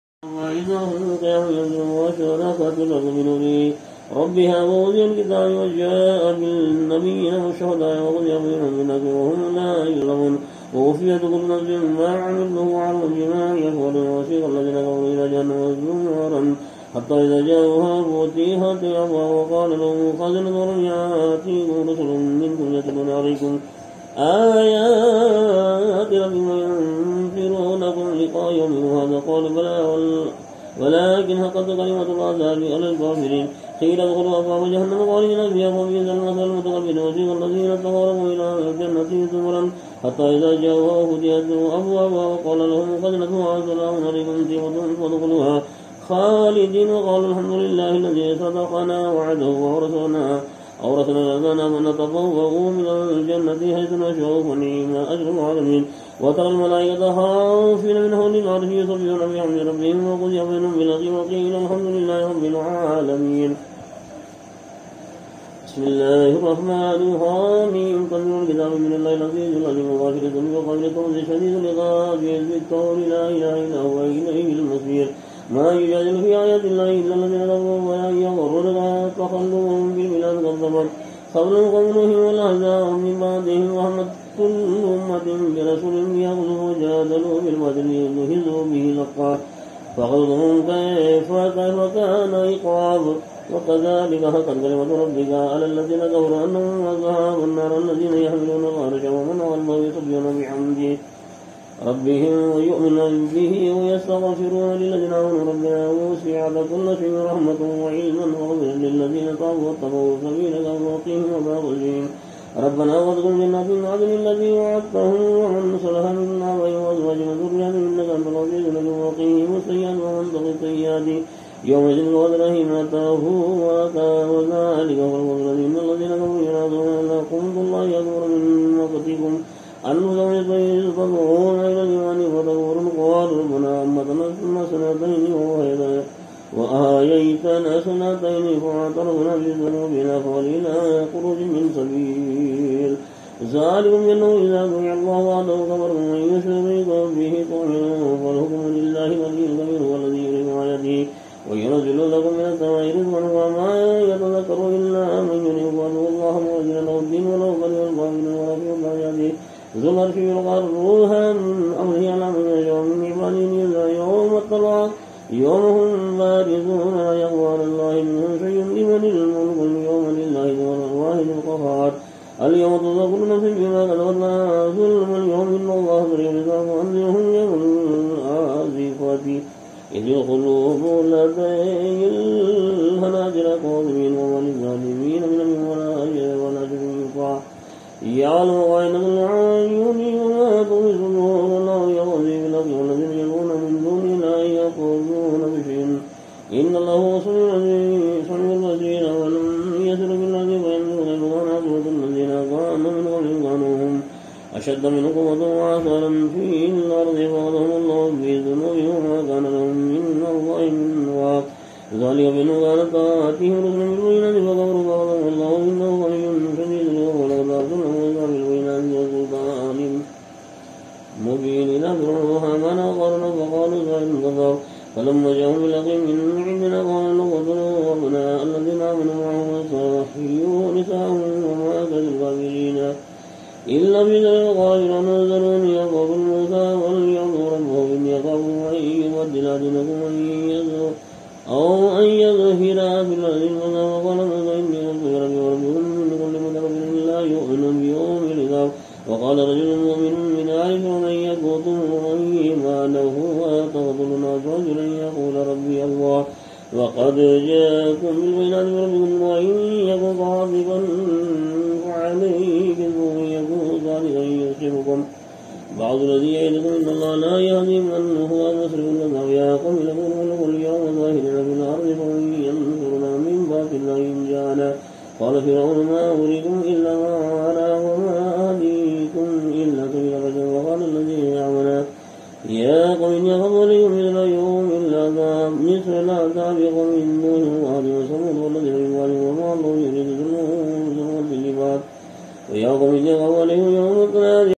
Quran reciting voice by : Alhaj Hazrat Syed shah murshad peer khadri urf kanekal saheb ( sufi )
Hazrat syed Murshad peer khadri saheb.  brother of Alhaj Hazrat Syed shah Abdul wahab khadri jaami &         S/O of Hazrat syed shah khader basha Khadri.  RA . &   G/so    of Hazrat syed shah Abdul wahab peer khadri RA urf Deewane peer saheb kanekal.     Aastanaye khadriya kanekal. (  AP )